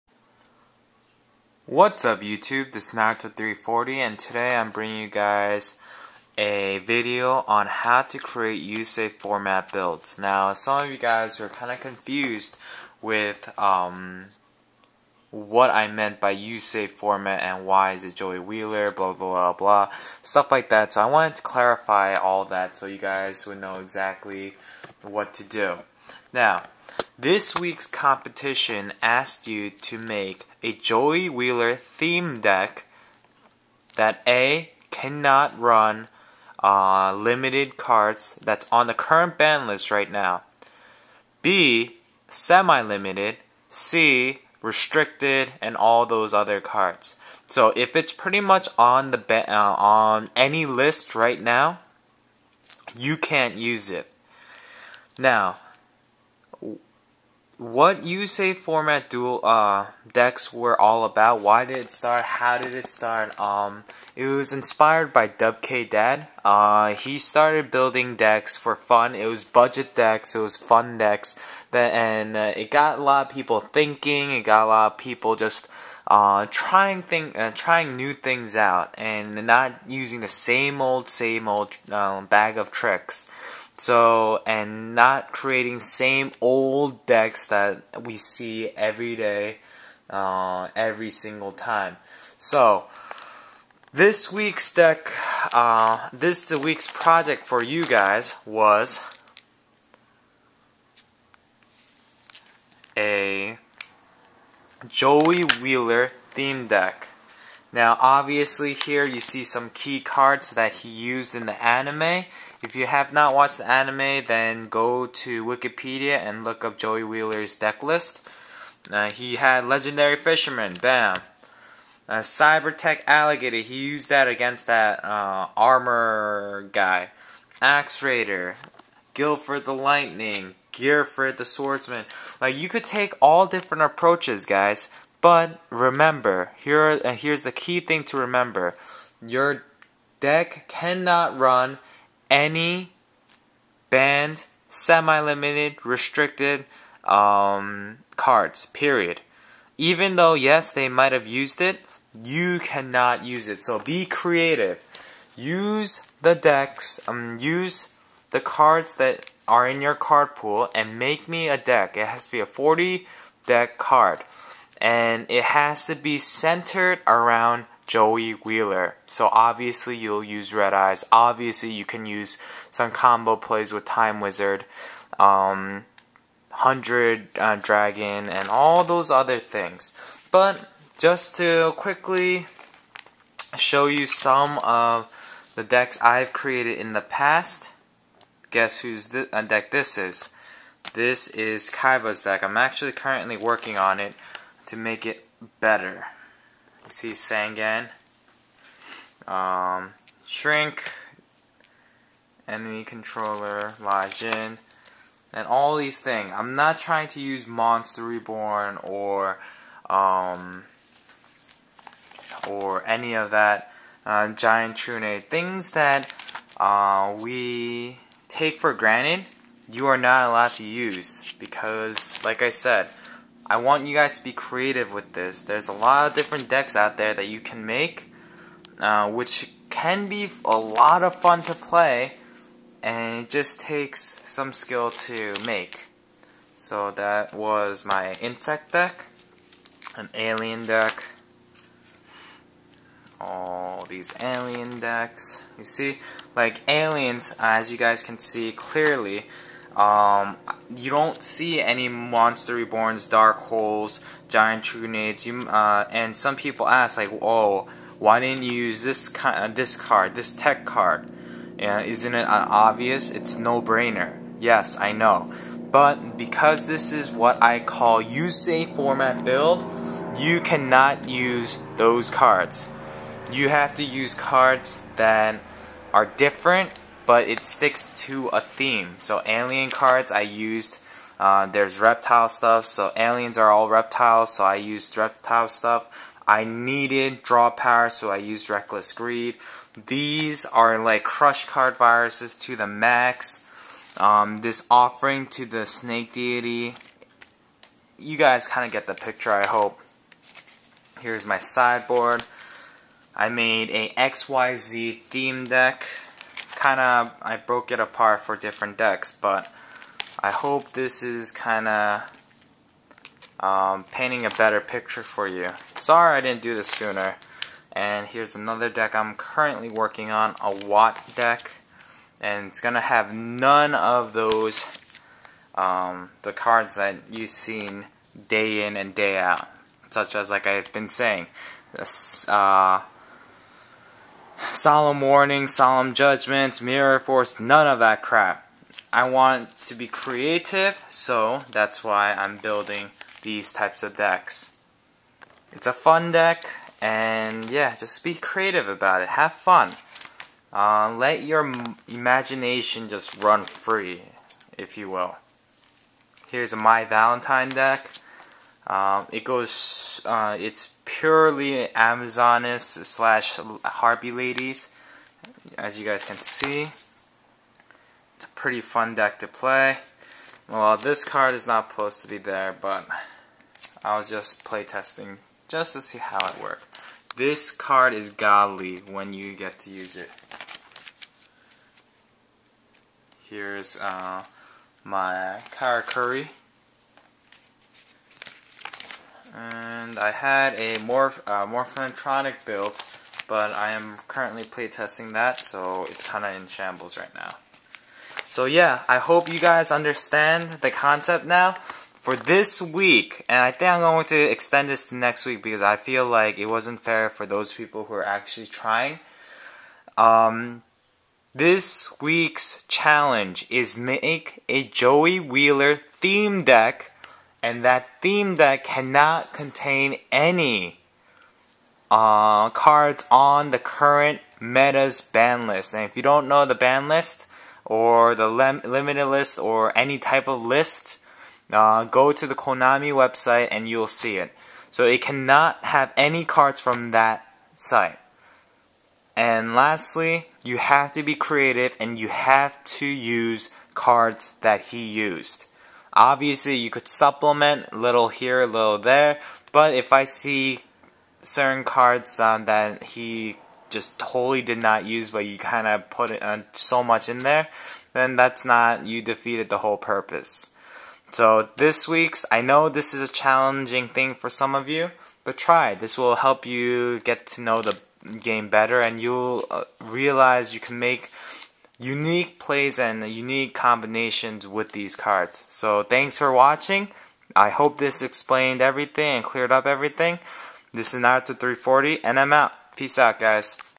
Yusei Format Deck Building Challenge!
Whats up guys! Today I am bringing you a video that clarifies some confusion about how Yusei Format Deck Building goes. If you you are interested in earning some points definitely try making these decks! Its fun, challenging and it helps you learn some mechanics and it helps you learn more about the different cards in the card pool.

This weeks competition revolves around

JOEY WHEELER!

RULES:
-Stick to the theme.
-You cannot use cards that are currently on the Semi limited list, Restricted List and the banned list.